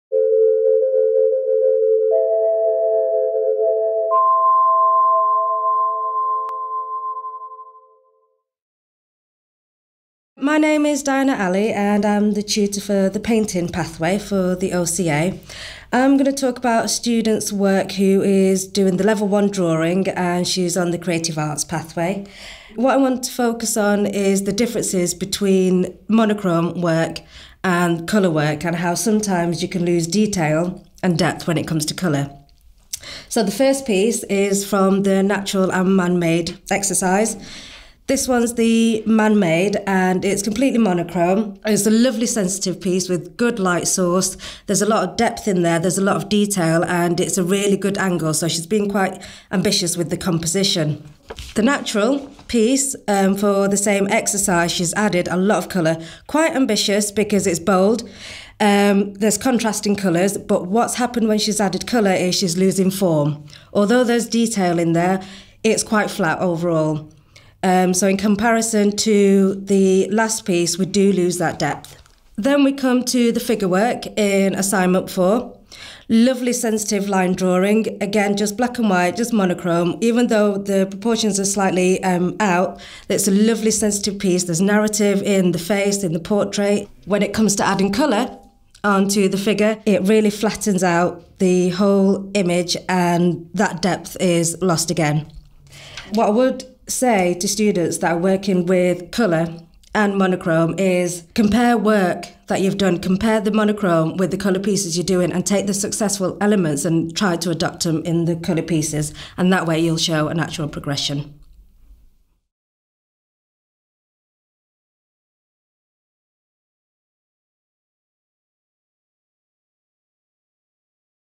Student work uncovered: Irena Stefania Plutasu
OCA tutor and assessor Diana Ali looks at the work of Drawing 1 student Irena Stefania Plutasu.